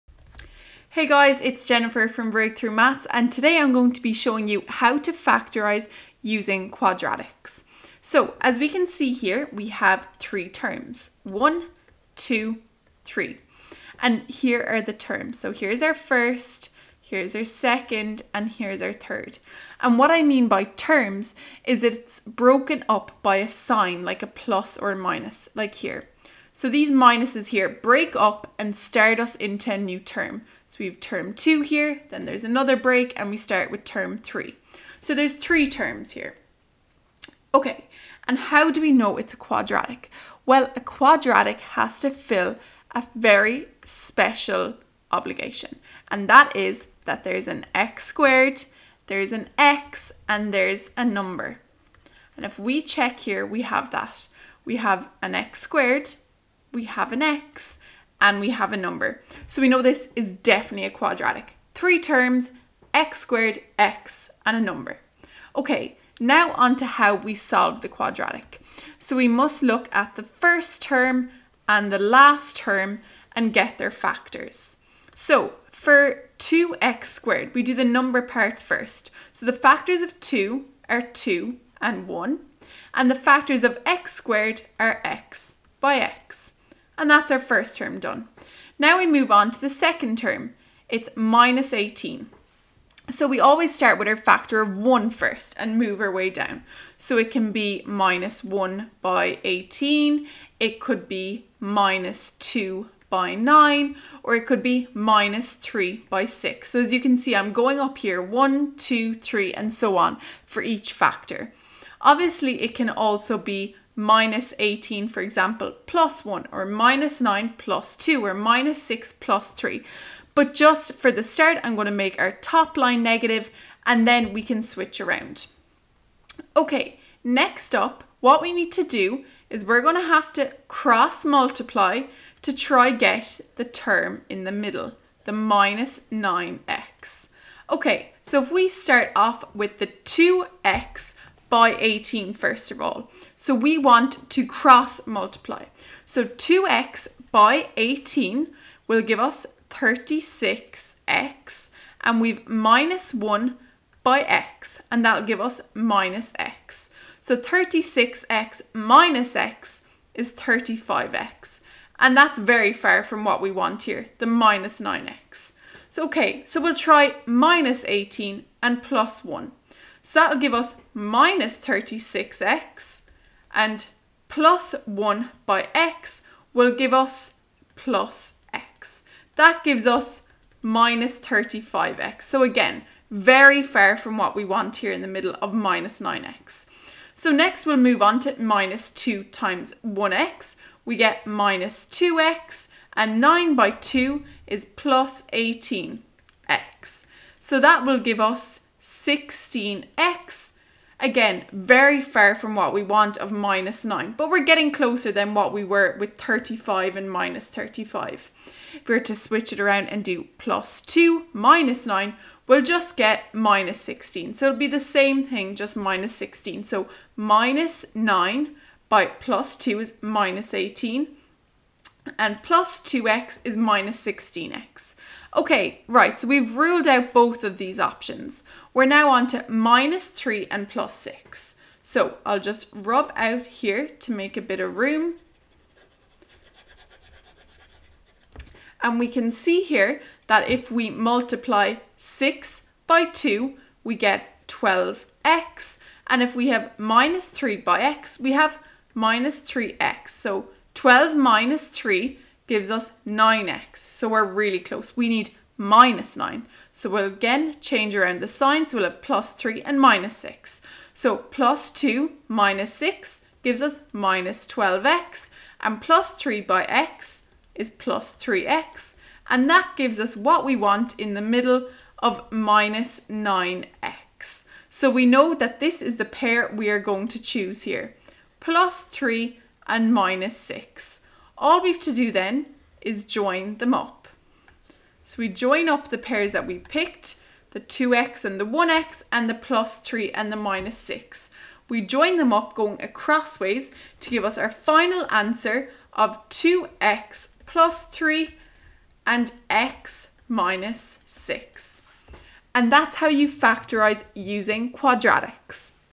A really simple guide to Quadratic Equations
This confuses so many students.

And it's so key to Maths.

You have to get how to do Quadratic Equations or Trinomials, as they say in the US.

This is a brilliant video on how to do simple, straightforward questions.